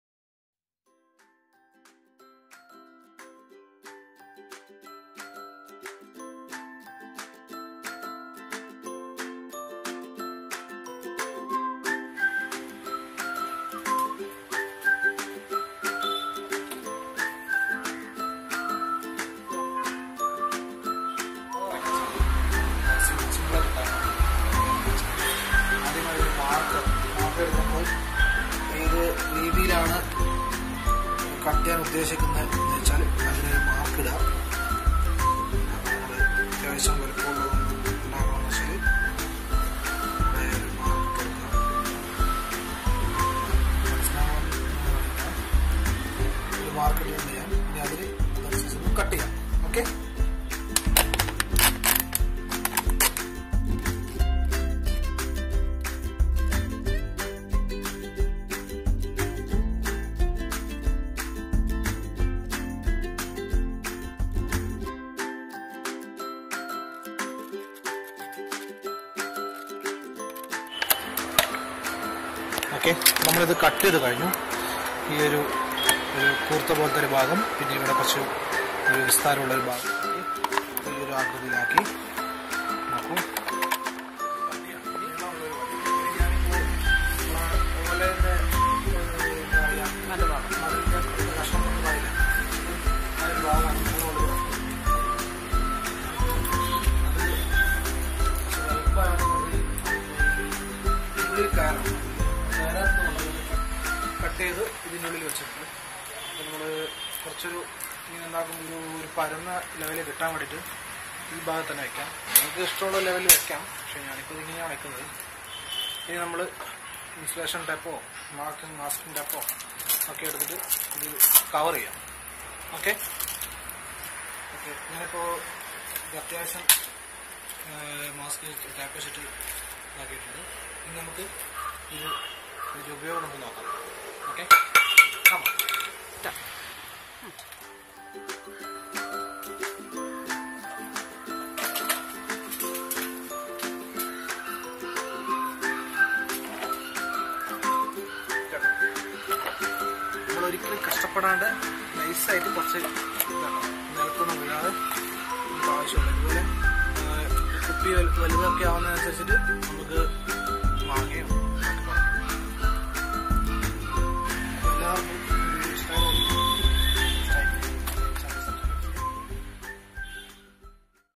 Ideas with Plastic Bottles
പാഴായ പ്ലാസ്റ്റിക്ക് ബോട്ടിൽ ഉപയോഗിച്ച് എങ്ങനെ ഒരു (കൊക്ക ) തോട്ടി ഉണ്ടാക്കാം '